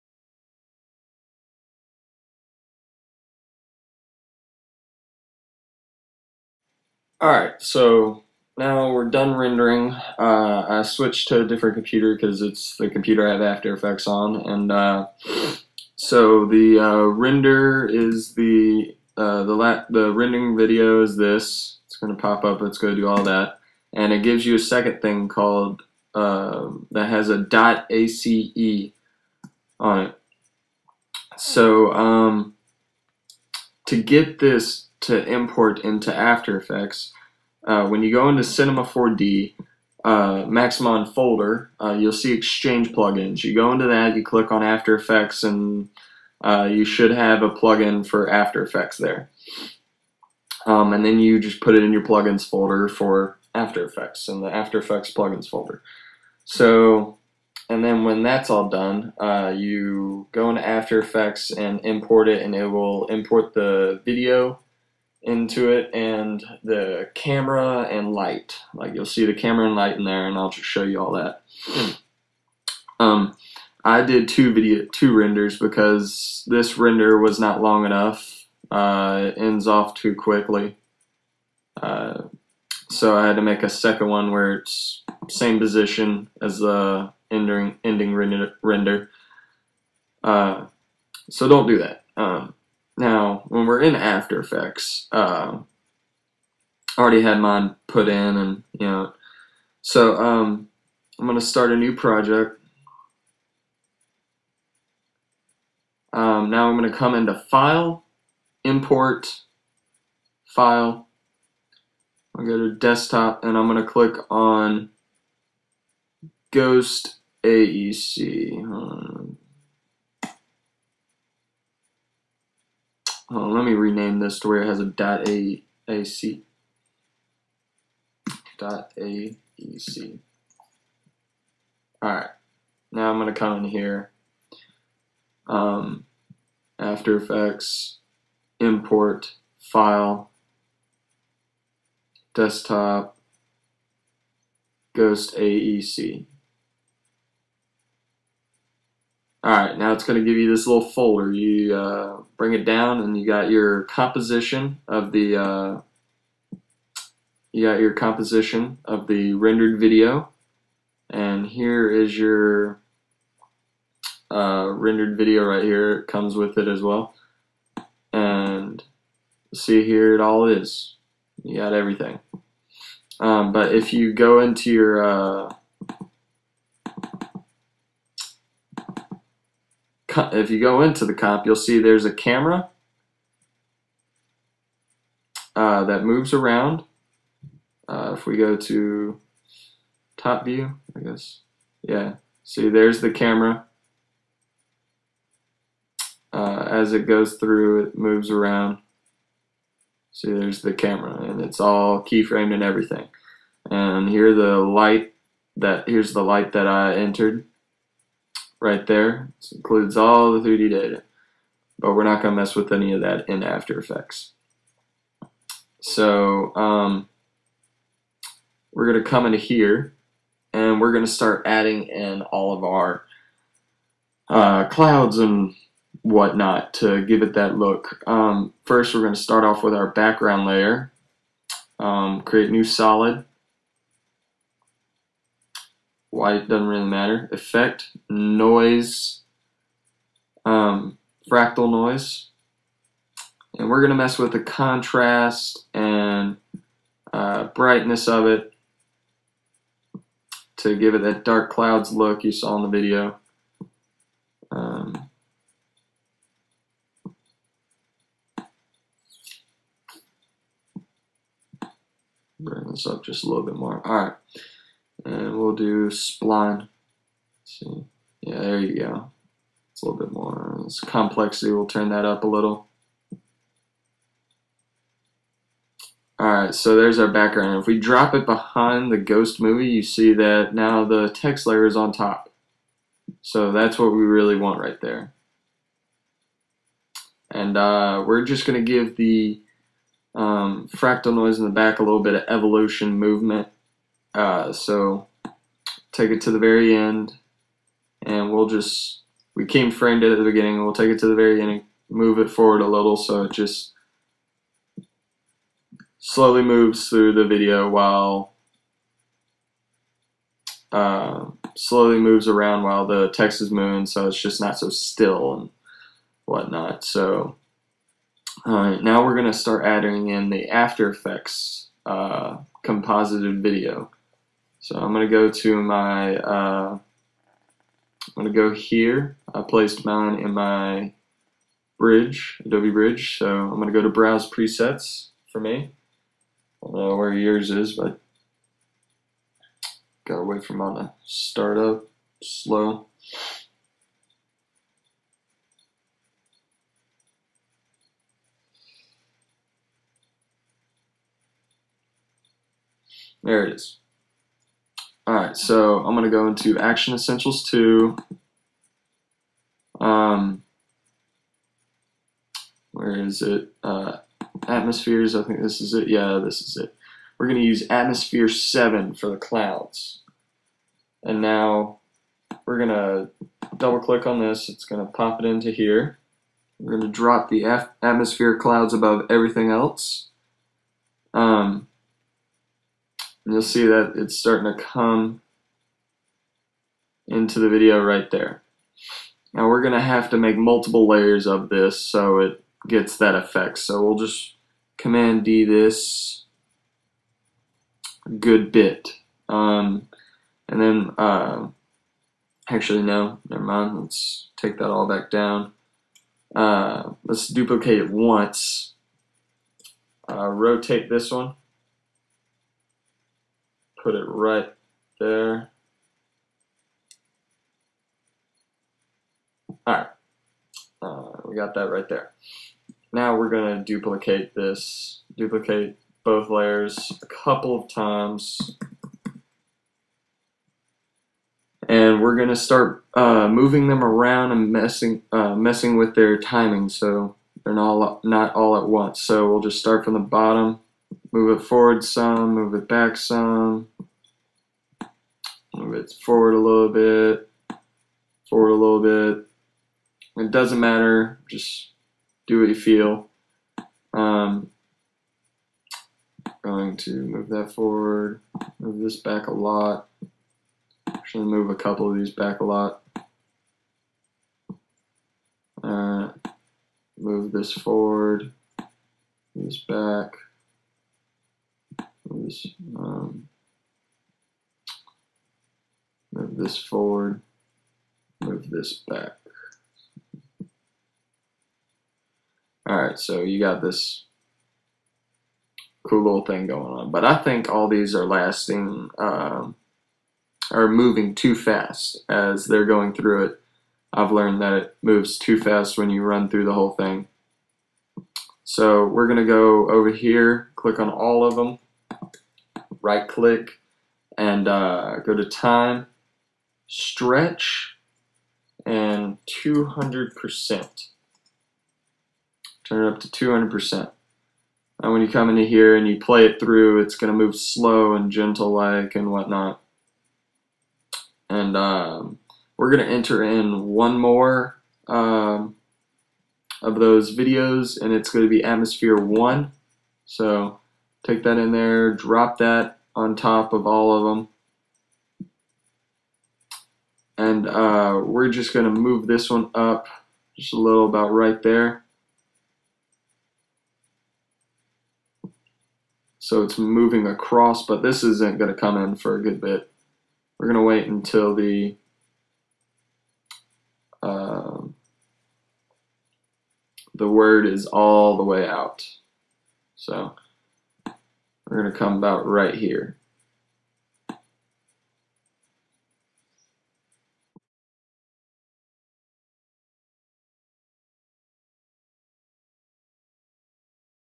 After Effects: Dark Cloud Tutorial Part3
how to put the render into after effects